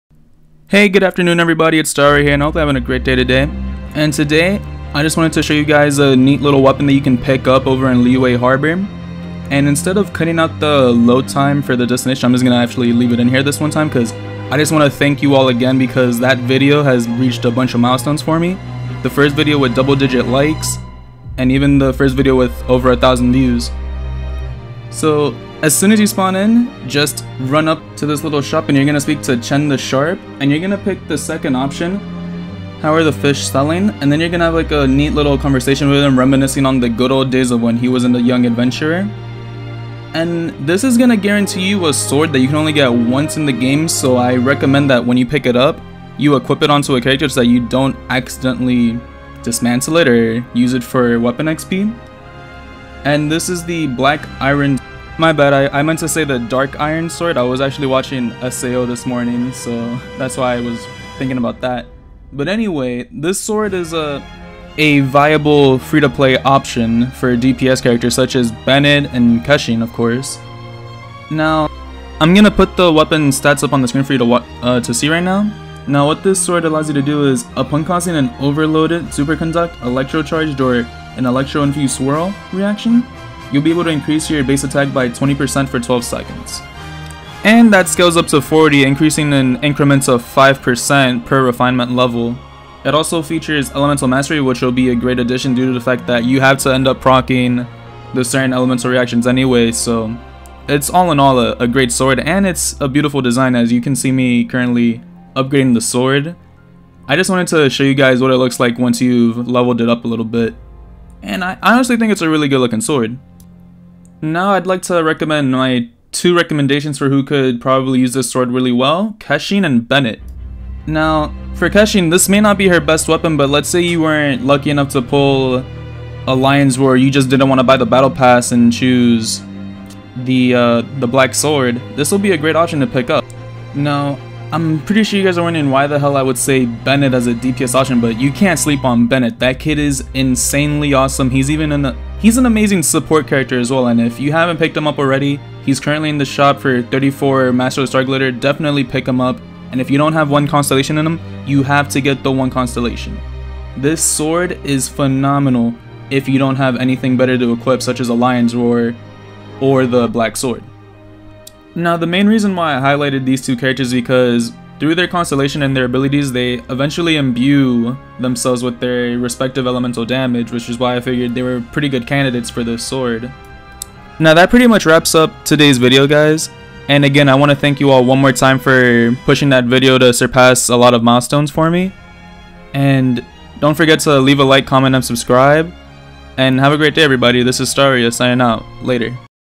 One Of A Kind SWORD. Did I Mention it's F2P
The Dark Iron Sword is a one of a kind and is not obtainable through any other means at the moment of recording. If you don't have a 4 or 5 Star sword this F2P sword is a great option. Also it'll look good in your Collection.